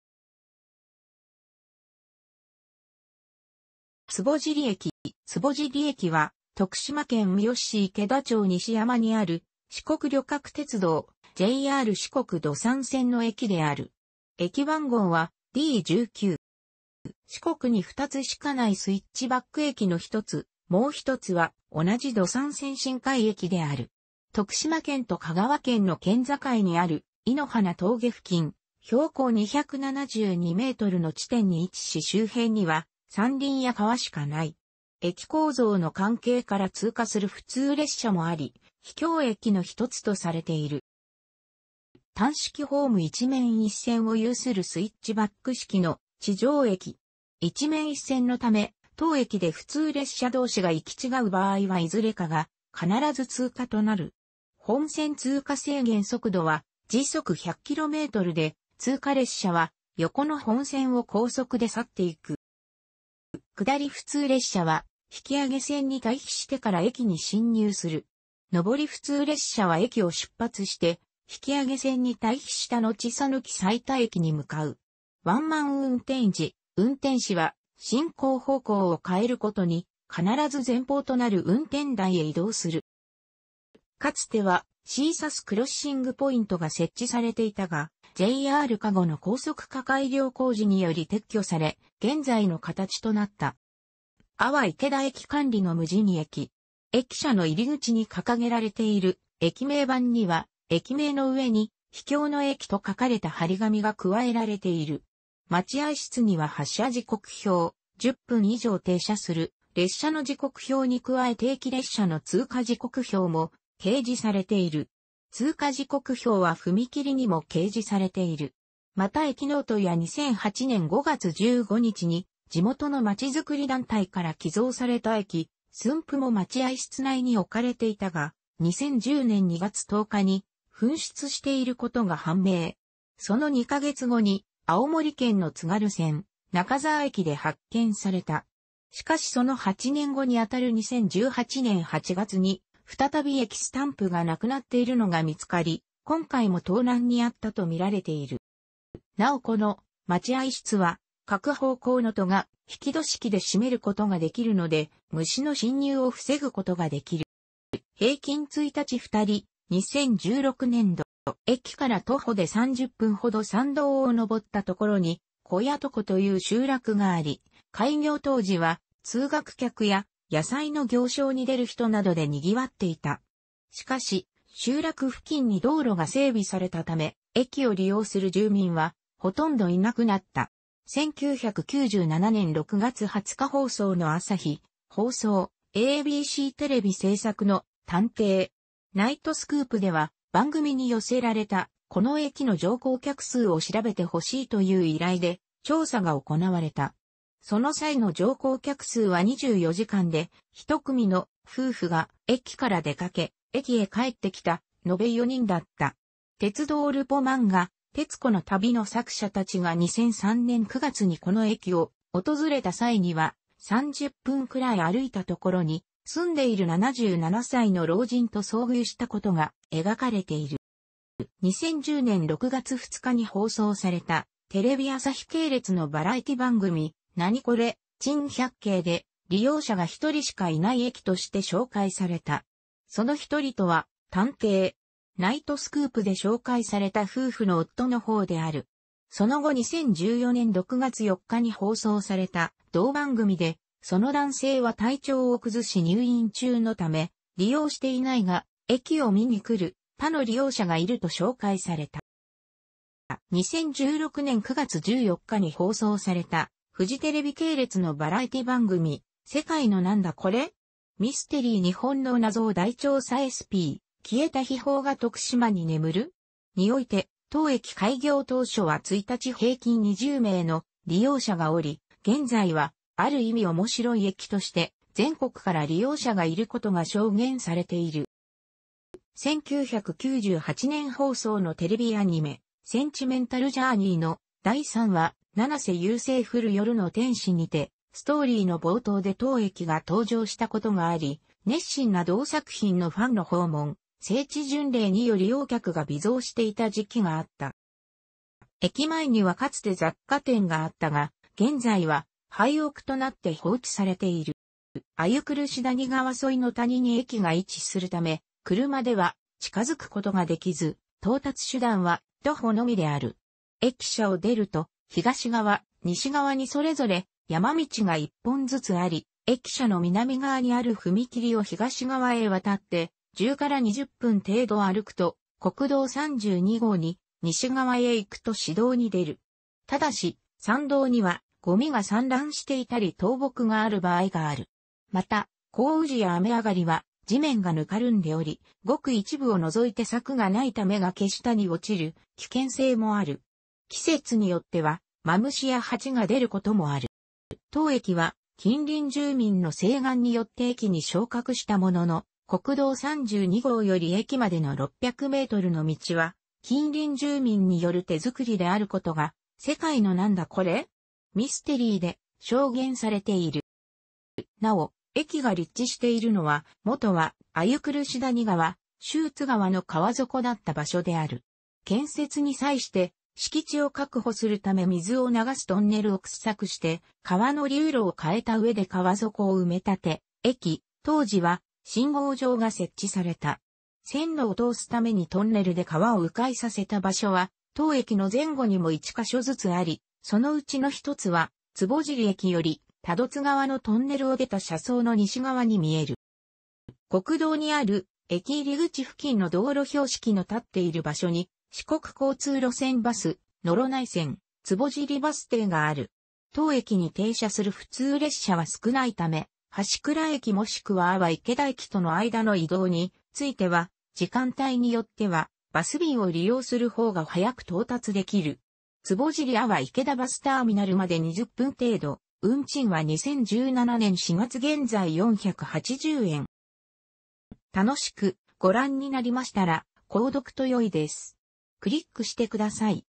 坪尻駅
坪尻駅（つぼじりえき）は、徳島県三好市池田町西山にある、四国旅客鉄道（JR四国）土讃線の駅である。駅番号はD19。

四国に2つしかないスイッチバック駅の1つ（もう1つは同じ土讃線新改駅）である。徳島県と香川県の県境にある猪ノ鼻峠付近、標高272mの地点に位置し周辺には山林や川しかない。駅構造の関係から通過する普通列車もあり、秘境駅のひとつとされている。

単式ホーム1面1線を有するスイッチバック式の地上駅。1面1線のため、当駅で普通列車同士が行き違う場合はいずれかが必ず通過となる。本線通過制限速度は100km/hで、通過列車は、横の本線を高速で去っていく。

下り普通列車は引き上げ線に待避してから駅に進入する。上り普通列車は駅を出発して引き上げ線に待避したのち讃岐財田駅に向かう。ワンマン運転時、運転士は進行方向を変えるごとに必ず前方となる運転台へ移動する。

かつてはシーサス・クロッシングポイントが設置されていたが、JR化後の高速化改良工事により撤去され、現在の形となった。

阿波池田駅管理の無人駅。駅舎の入口に掲げられている駅名板には駅名の上に『秘境の駅』と書かれた貼り紙が加えられている。待合室には発車時刻表、10分以上停車する列車の時刻表に加え定期列車の通過時刻表も掲示されている。通過時刻表は踏切にも掲示されている。また駅ノートや2008年5月15日に地元の町づくり団体から寄贈された駅スタンプも待合室内に置かれていたが、2010年2月10日に紛失していることが判明。その２ヶ月後に青森県の津軽線中沢駅で発見された。しかしその8年後に当たる2018年8月に、再び駅スタンプが無くなっているのが見つかり、今回も盗難に遭ったと見られている。

なおこの待合室は各方向の戸が引き戸式で閉めることができるので、虫の侵入を防ぐことができる。

平均1日2人（2016年度）

駅から徒歩で30分ほど山道を登った所に木屋床（こやとこ）という集落があり、開業当時は通学客や野菜の行商に出る人などで賑わっていた。しかし、集落付近に道路が整備されたため、駅を利用する住民はほとんど居なくなった。
1997年6月20日放送の朝日放送（ABCテレビ）製作の『探偵!ナイトスクープ』では、番組に寄せられた「この駅の乗降客数を調べてほしい」という依頼で、調査が行われた。その際の乗降客数は24時間で、一組の夫婦が駅から出かけ、駅へ帰ってきた「のべ4人」だった。鉄道ルポ漫画『鉄子の旅』の作者達が2003年9月にこの駅を訪れた際には、「30分くらい歩いたところに」住んでいる77歳の老人と遭遇したことが描かれている。

2010年6月2日に放送されたテレビ朝日系列のバラエティ番組『ナニコレ珍百景』で、「利用者が1人しか居ない駅」として紹介された。その1人とは『探偵!ナイトスクープ』で紹介された夫婦の夫の方である。その後2014年6月4日に放送された同番組で、その男性は体調を崩し入院中のため利用していないが、駅を見にくる他の利用者がいると紹介された。

2016年9月14日に放送されたフジテレビ系列のバラエティ番組『世界の何だコレ!?ミステリー 日本の謎を大調査SP/消えた秘宝が徳島に眠る?』において、当駅開業当初は1日平均20名の利用者がおり、現在はある意味面白い駅として、全国から利用者がいることが証言されている。

1998年放送のテレビアニメ『センチメンタルジャーニー』の第3話「七瀬優 星降る夜の天使」にてストーリーの冒頭で当駅が登場したことがあり、熱心な同作品のファンの訪問（聖地巡礼）により利用客が微増していた時期があった。

駅前にはかつて雑貨店があったが、現在は廃屋となって放置されている。

鮎苦谷川沿いの谷に駅が位置するため、車では近づくことができず、到達手段は徒歩のみである。駅舎を出ると、東側・西側にそれぞれ山道が1本ずつあり、駅舎の南側にある踏切を東側へ渡って10 - 20分程度歩くと国道32号に、西側へ行くと市道に出る。ただし、山道にはゴミが散乱していたり倒木がある場合がある。また、降雨時や雨上がりは地面がぬかるんでおり、ごく一部を除いて柵がないため崖下に落ちる危険性もある。季節によってはマムシやハチが出ることもある。

当駅は近隣住民の請願によって駅に昇格したものの、国道32号より駅までの600mの道は、近隣住民による手作りであることが『世界の何だコレ!?ミステリー』で...